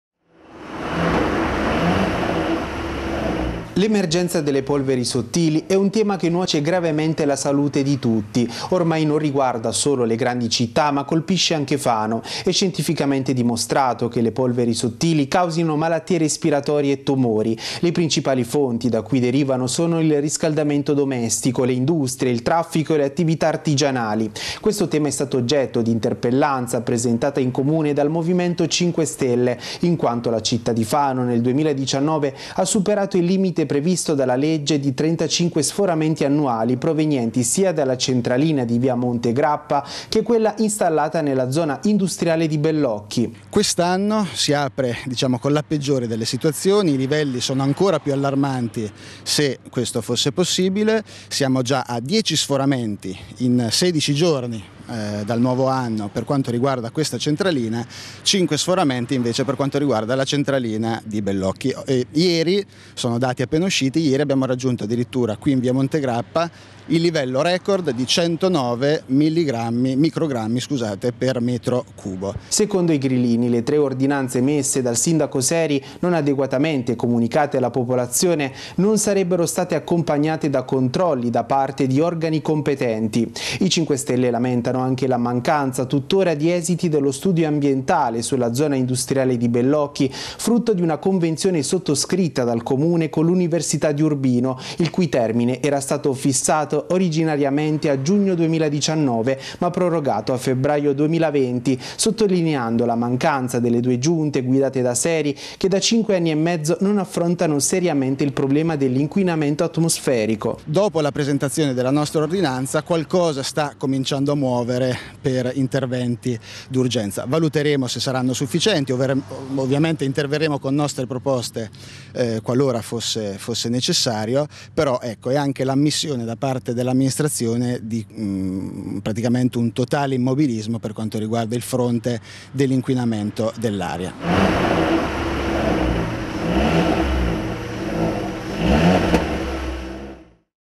M5S Fano presenta interpellanza su polveri sottili
Inquinamento atmosferico: sono preoccupanti i dati diffusi dall’Arpam. Il movimento cinque stelle di Fano chiede interventi urgenti per fronteggiare l’emergenza delle polveri sottili.